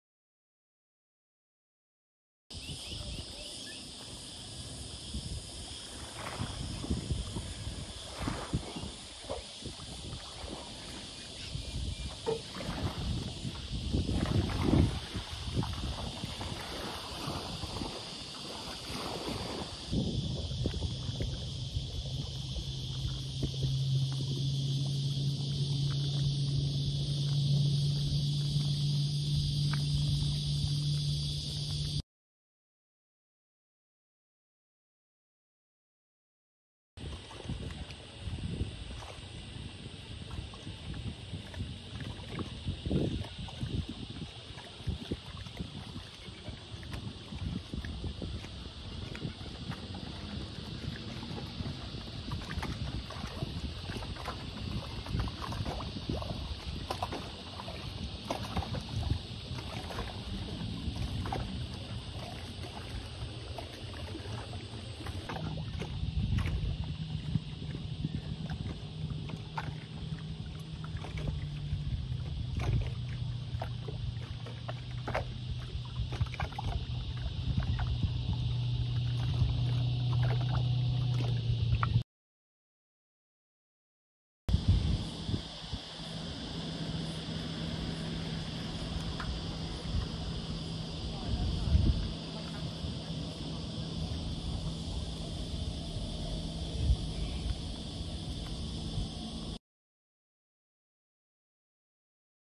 13 January 2024
High tide at Woy Woy Bay and Phegans Bay, Central Coast. Spike Milligan Bridge. Woy Woy Train Station.